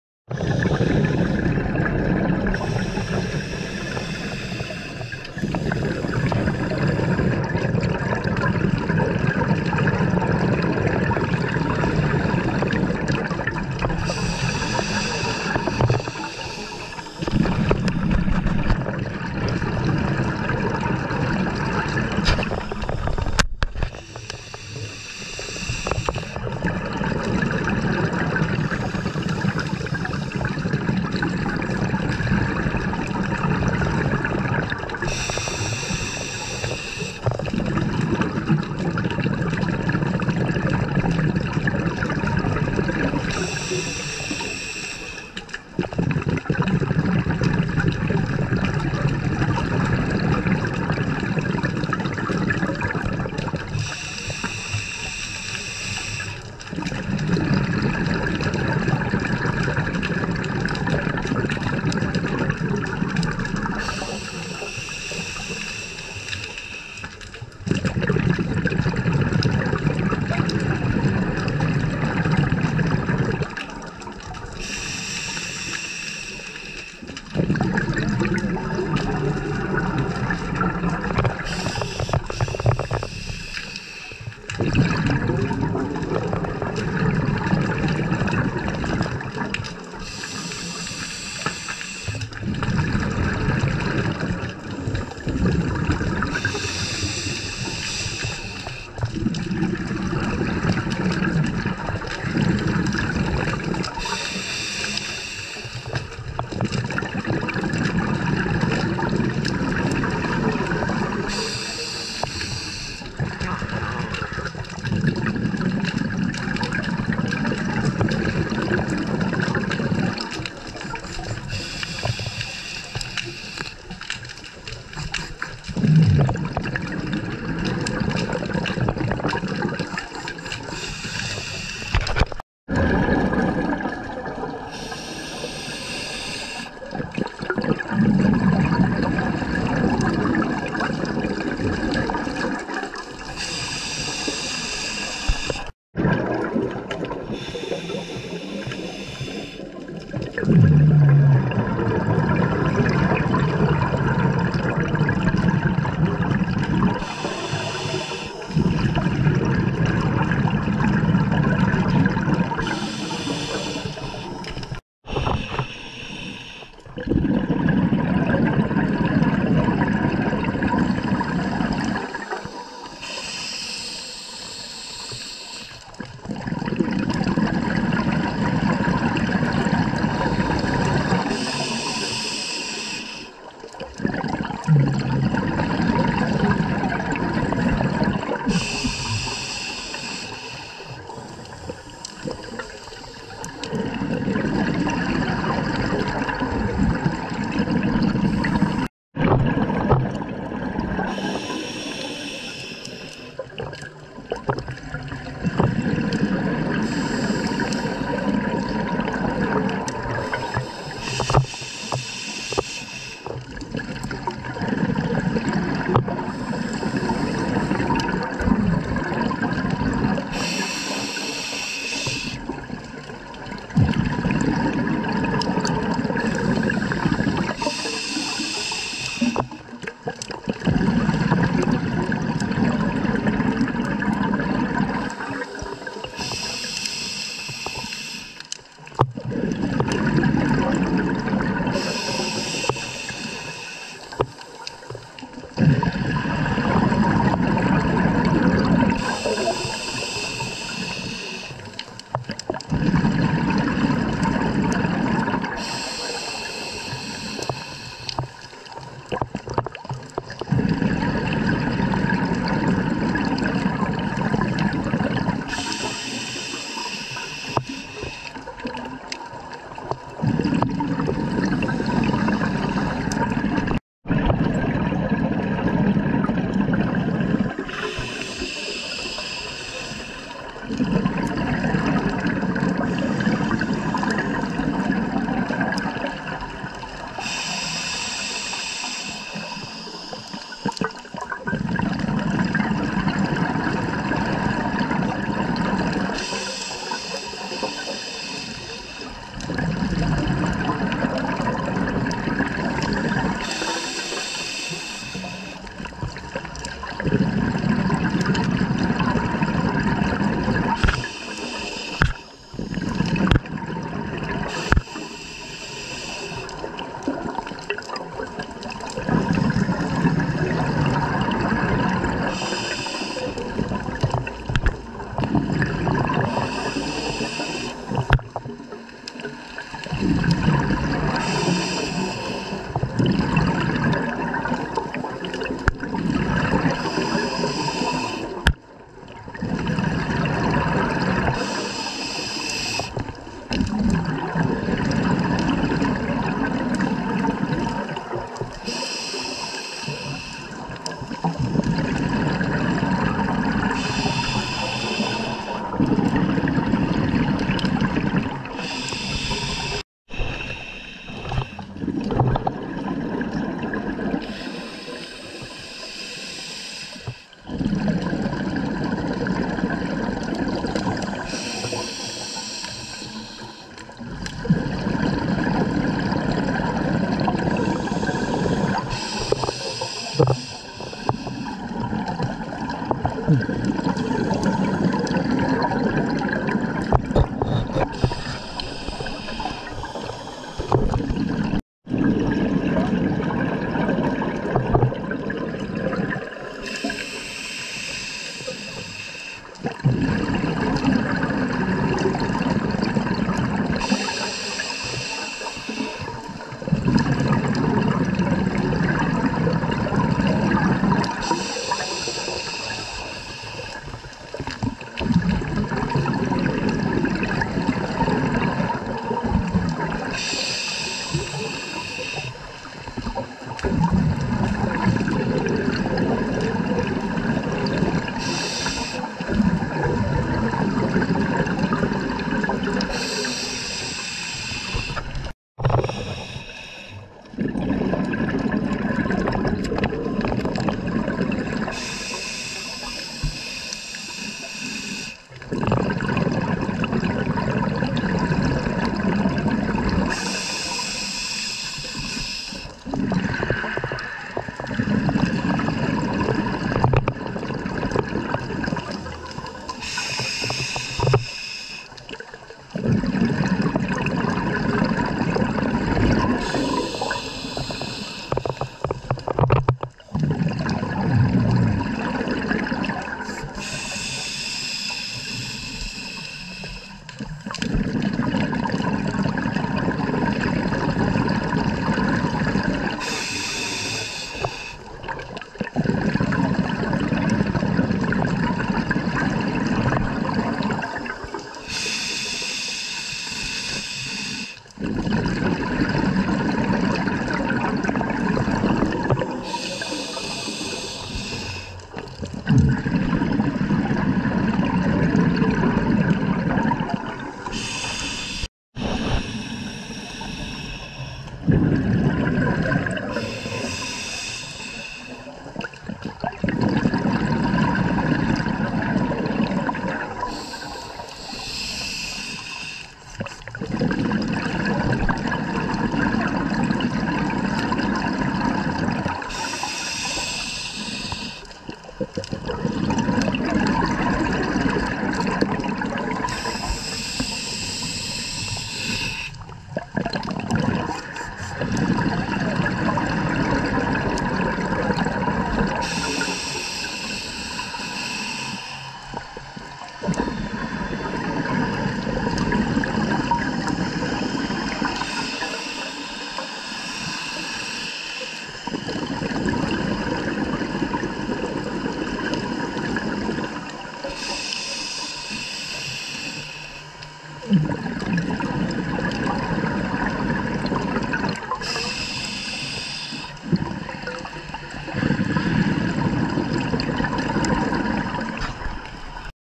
Diving the North Wall Tassie 2022
This is when we dived the North wall in Pirates Bay, Eaglehawk Neck Tasmania through Eaglehawk Dive Centre.

As the video shows it was covered in sealife with all sorts of fish and even reports of Port Jackson sharks which have nested there. However we didnt see any though.

I did have one little angry fish try to attach the camera at the 5:00 minute mark which was funny to see.

We got to a max of about 25m in depth however on this wall dive you can go from 18m down to about 38m